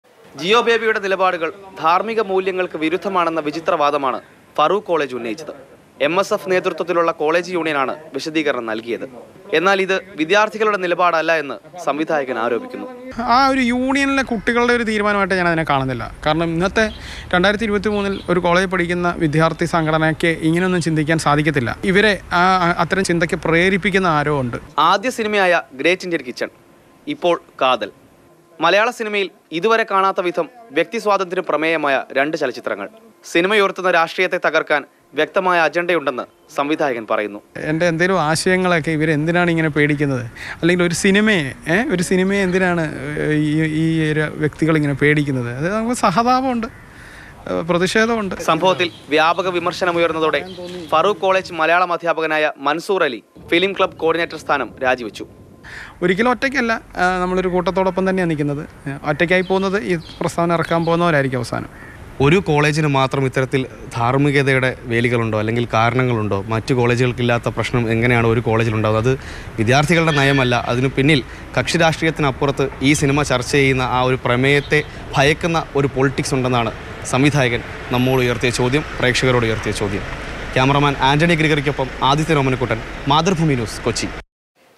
അതിഥിയായി ക്ഷണിച്ചു ശേഷം പരിപാടി റദ്ദാക്കി; ഫാറൂഖ് കോളേജിനെതിരെ പ്രതിഷേധവുമായി ജിയോബേബി
Happy viewing!